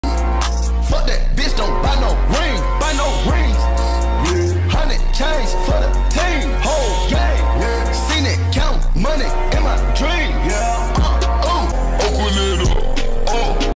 KELDON JOHNSON GOT HYPED AFTER BLOCK DONOVAN MITCHEL
SPORTFANBASE

THE PAGE IS ALLL ABOUT ANY SPORTS

BASKETBALL IS LIFE 🏀
VIDEO EDITOR

FOLLOW & SUPPORT